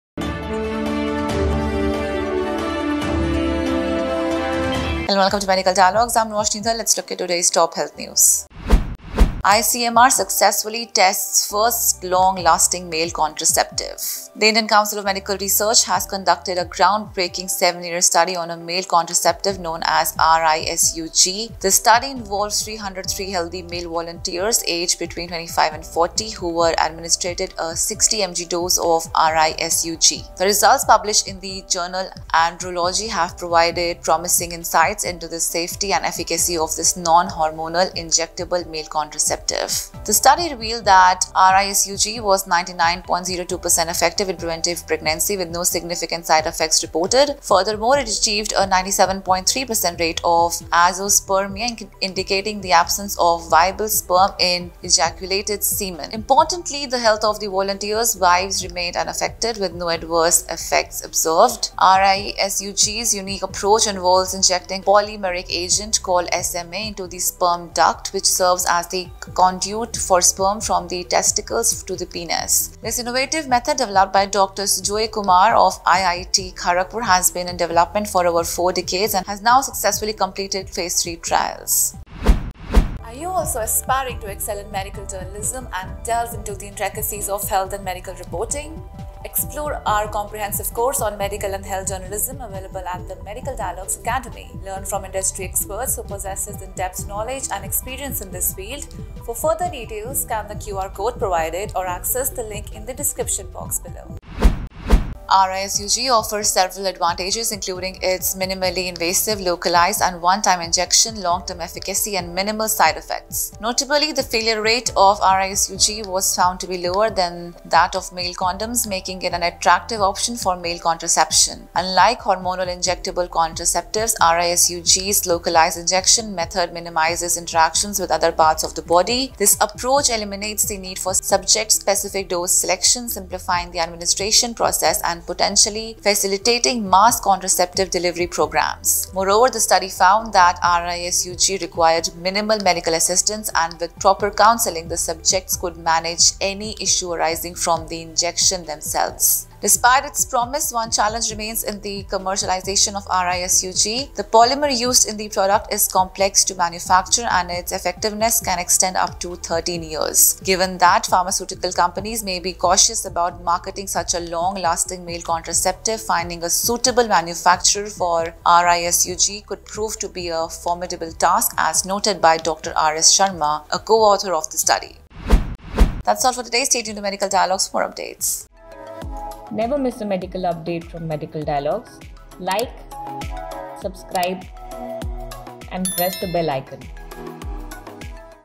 ICMR successfully tests first long-lasting male contraceptive
The Indian Council of Medical Research (ICMR) has conducted a groundbreaking seven-year study on a male contraceptive known as RISUG (Reversible Inhibition of Sperm under Guidance). This study involved 303 healthy male volunteers aged 25-40, who were administered a 60 mg dose of RISUG. The results, published in the journal Andrology, have provided promising insights into the safety and efficacy of this non-hormonal, injectable male contraceptive. The study revealed that RISUG was 99.02% effective in preventing pregnancy, with no significant side effects reported. Furthermore, it achieved a 97.3% rate of azoospermia, indicating the absence of viable sperm in ejaculated semen. Importantly, the health of the volunteers' wives remained unaffected, with no adverse effects observed.